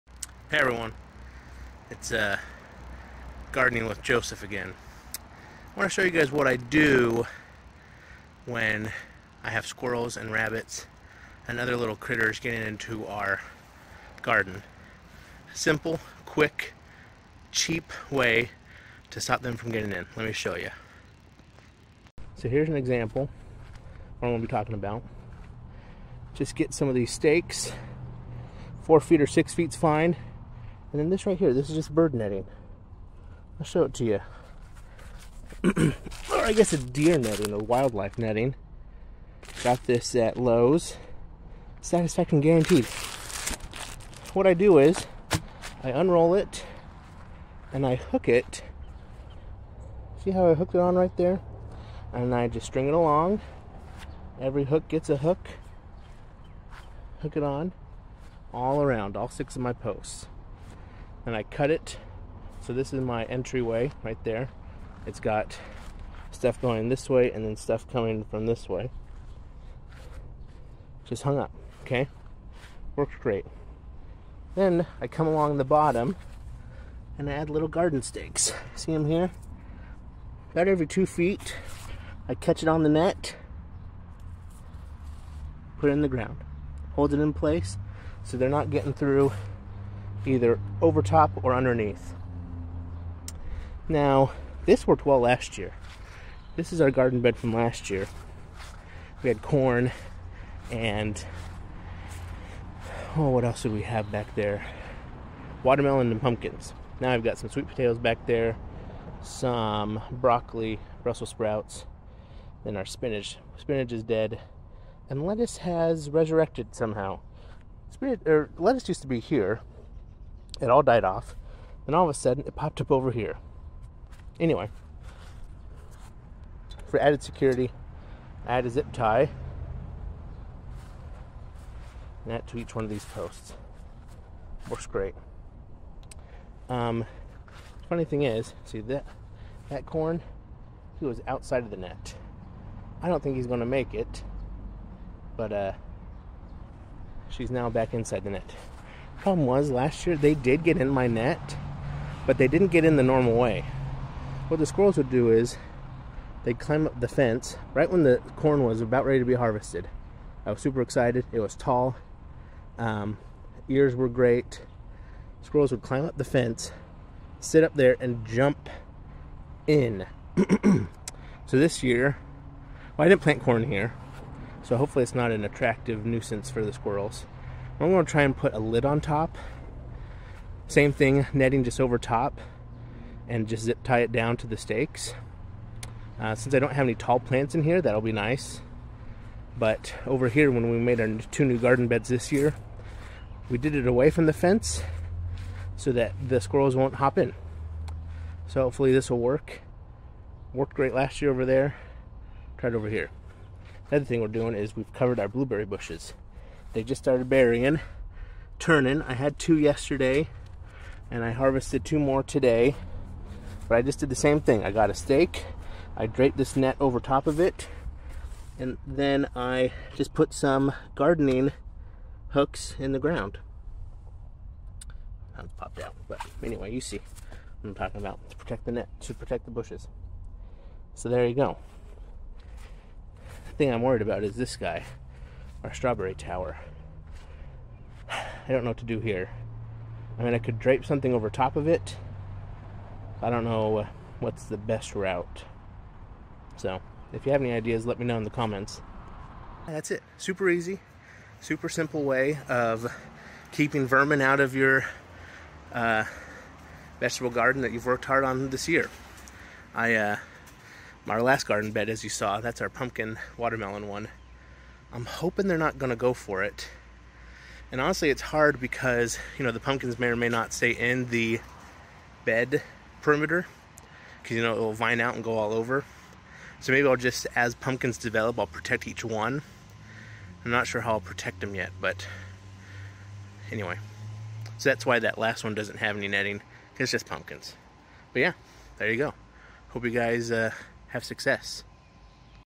How to Keep Squirrels and Rabbits out of Your Veggie Garden (CHEAP, EASY, EFFECTIVE)
By using just a few stakes, wildlife netting, and garden stakes you can keep your vegetable garden vermin free for no hassle.